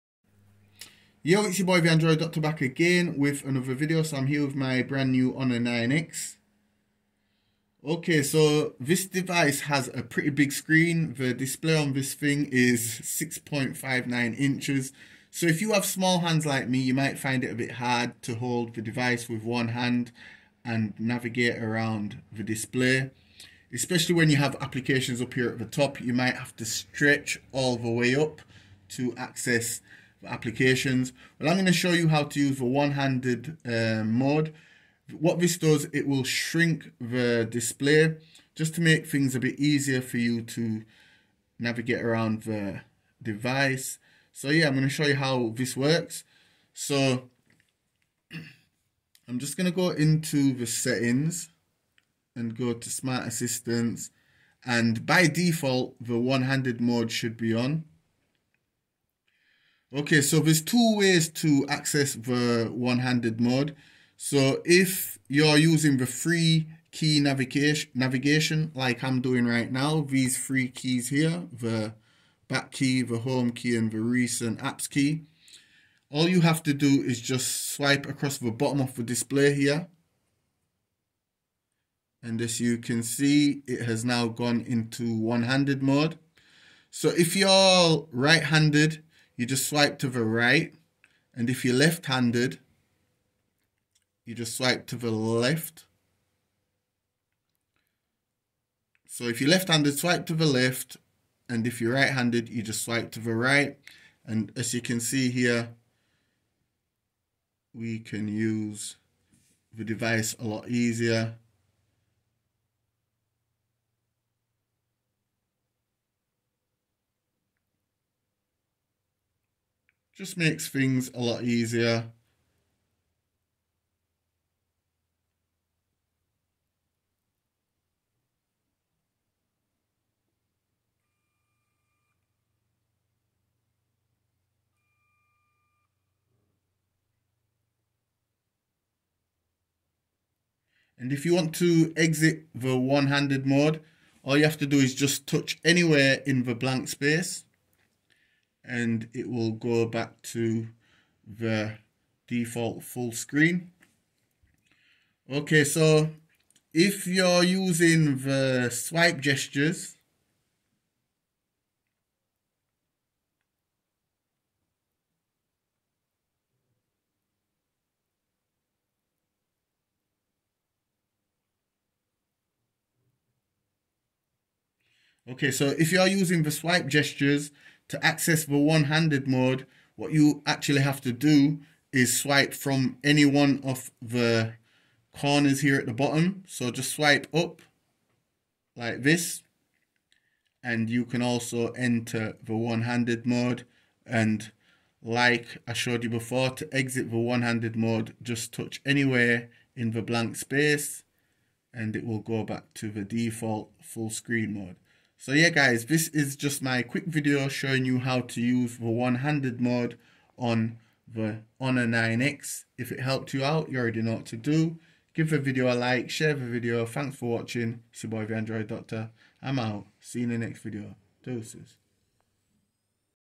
Honor 9X One Handed Mode Minimize The Screen Size For Easier Navigation Using 1 Hand
Honor 9X One Handed Mode Minimize The Screen For Easier Navigation Using 1 Hand